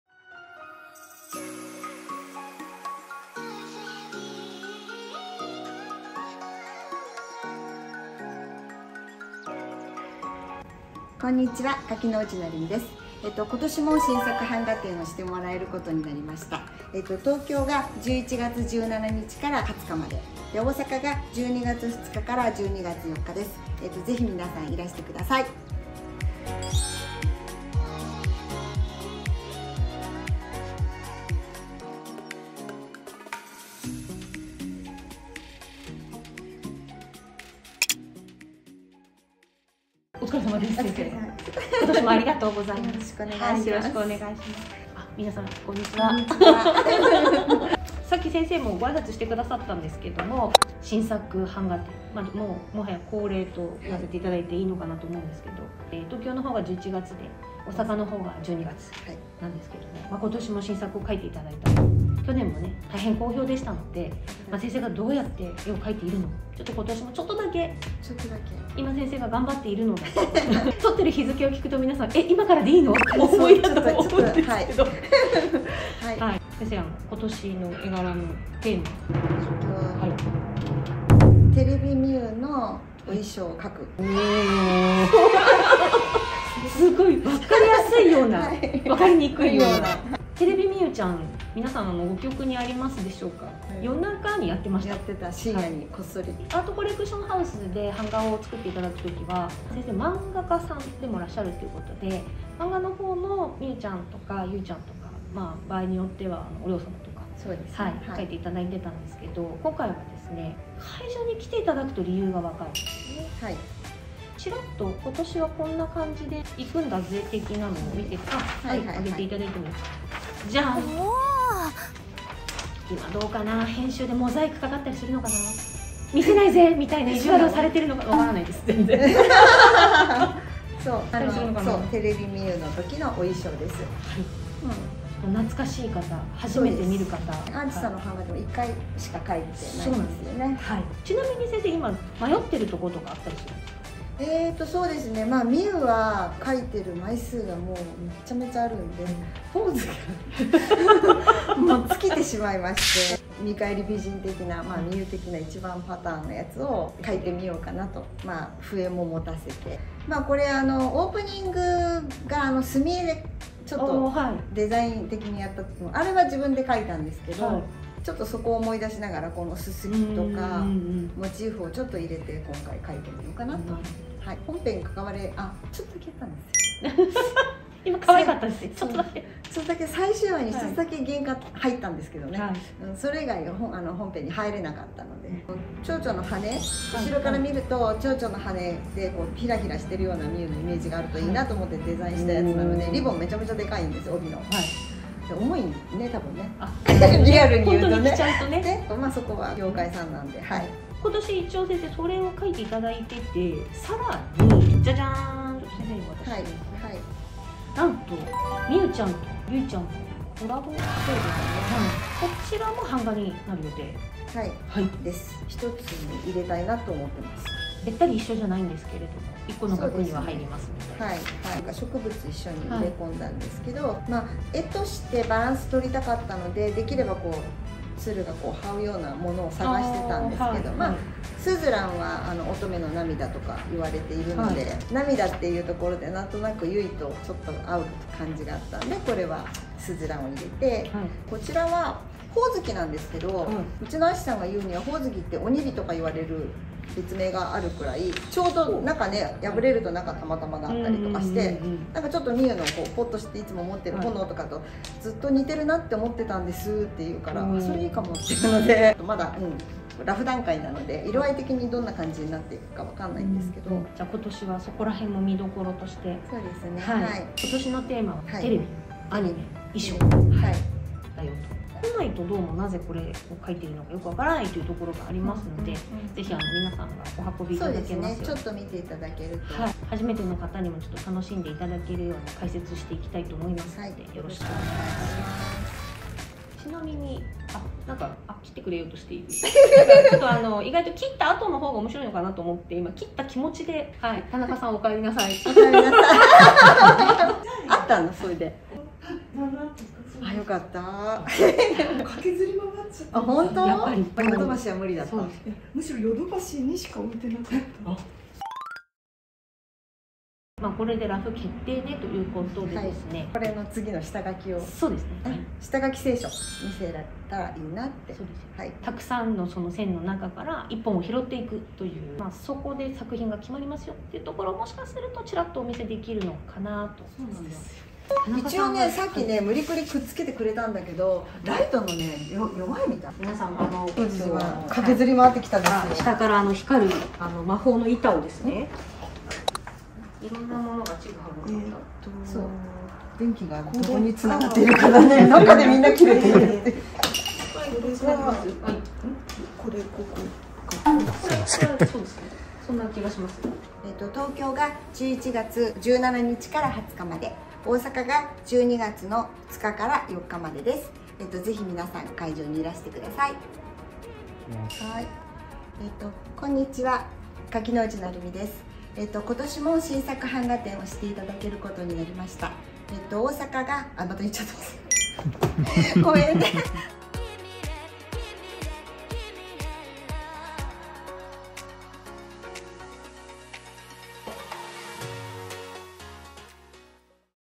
【インタビュー】漫画家・アニメーター 垣野内成美〜新作版画展2022 開催決定！！直前インタビュー |ENG SUB
※訂正
2:42
『オープニングでスミレのデザインを描いたときに』
正しくは
『エンディングで墨絵のデザインを描いたときに』
です🙏

『吸血姫美夕』『薬師寺涼子の怪奇事件簿』
など数々の人気漫画を描き
様々な作品の作画監督やキャラクターデザイン等
幅広く活動し続ける、垣野内 成美 先生

毎年多くのお客様にご来場頂いている、新作版画展の開催が今年も決定！！

【垣野内成美 新作版画展２０２２】

について垣野内先生に開催直前インタビュー。

出展予定の作品の『ラフ画』をご紹介しながら
今年の作品のテーマや、こだわりについて伺います。

今回の新作版画展は、皆様の予想を覆す内容となっているはず、、、✨
見応えのある内容となっております。
是非、会場にて作品をお楽しみください♪

東京会場
２０２２年１１月１７日（木）〜１１月２０日（日）
１１：００〜１９：００（最終日１７：００閉場）
開催会場・ギャラリー エピキュート

大阪会場
２０２２年１２月２日（金）〜１２月４日（日）
１１：００〜１９：００（初日１４：００開場、最終日１７：００閉場）
開催会場・ブリーゼプラザ　８０５

東京、大阪共にトークショウ&サイン会開催日あり

新作版画展、トークショウ&サイン会の詳細はこちらからご確認をお願い致します。
垣野内成美 新作版画展２０２２　特設サイト
（昨年の情報とお間違いがない様、お気をつけください）

引用画像
インターネット 花キューピット